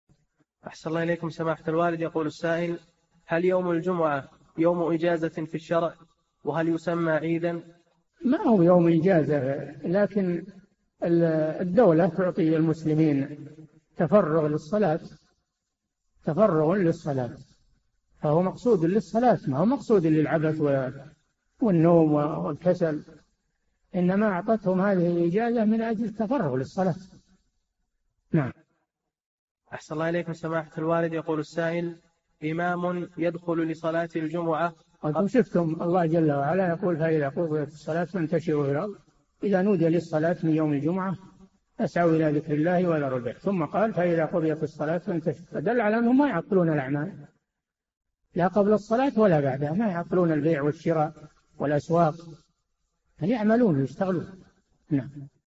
[1860- 4717] هل يوم الجمعة يوم إجازة في الشرع وهل يسمى عيداً؟ - الشيخ صالح الفوزان
[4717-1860] هل يوم الجمعة يوم إجازة في الشرع؟ وهل يسمى عيدًا؟
الجَوَابُ: ما هو يوم إجازة لكن الدولة تعطي المسلمين التفرغ للصلاة فهو مقصود للصلاة ما هو مقصود للعبث والنوم والكسل، إنما أعطته هذه الإجازة من أجل التفرغ للصلاة، والله جل وعلا يقول فإذا قضيت الصلاة فانتشروا في الأرض، ﴿إِذَا نُودِيَ لِلصَّلَاةِ مِنْ يَوْمِ الْجُمُعَةِ فَاسْعَوْا إِلَى ذِكْرِ اللَّهِ وَذَرُوا الْبَيْعَ﴾ ثم قال: ﴿فَإِذَا قُضِيَتِ الصَّلَاةُ فَانْتَشِرُوا فِي الْأَرْضِ ﴾ [الجمعة: 9، 10] فدل على أنهم ما يعطلون الأعمال، لا قبل الصلاة ولا بعدها ما يعطلون البيع والشراء والأسواق بل يعملون ويشتغلون.
السيرة الذاتية :
للاشتراك في قناة الشيخ صالح الفوزان
لمشاهدة حلقات فتاوى على الهواء كاملة

للاستفسار والإقتراحات والدعم
رئيس الفريق
لمشروع كبار العلماء